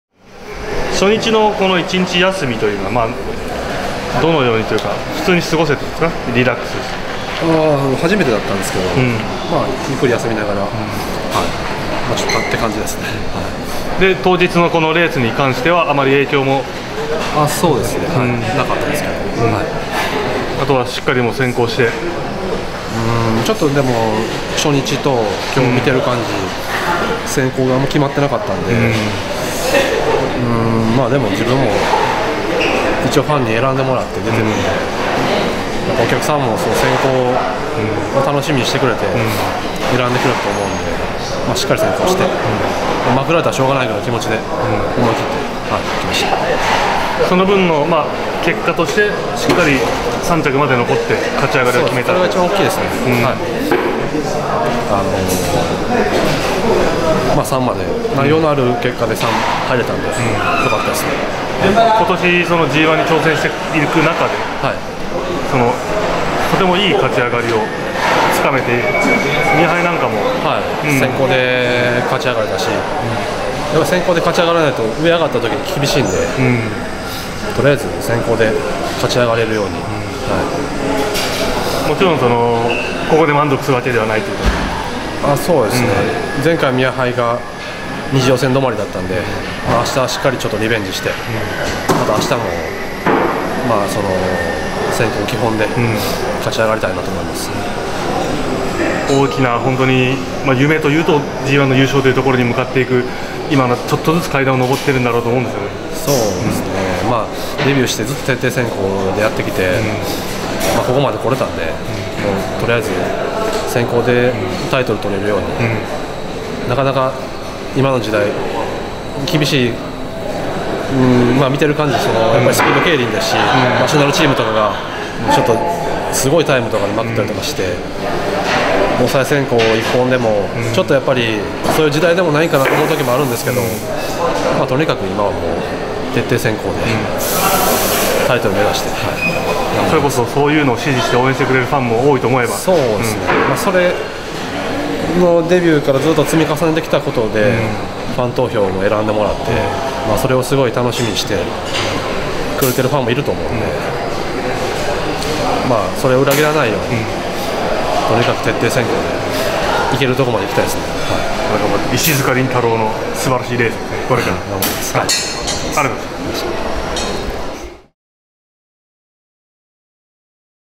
【ＧⅠオールスター競輪】“紀州の大車輪”石塚輪太郎がド迫力の逃げ披露
２日目の一次予選７Ｒを走った石塚輪太郎（２４）は、果敢に風を切って３着に粘り込んだ。デビューから先行にこだわって走ってきて、その泥臭い走りはファンの支持を集めている。オールスターだからこそ、ファンのために先行を、の気持ちは強まるばかりだ。石塚輪太郎は昭和の競輪道を歩んでいる。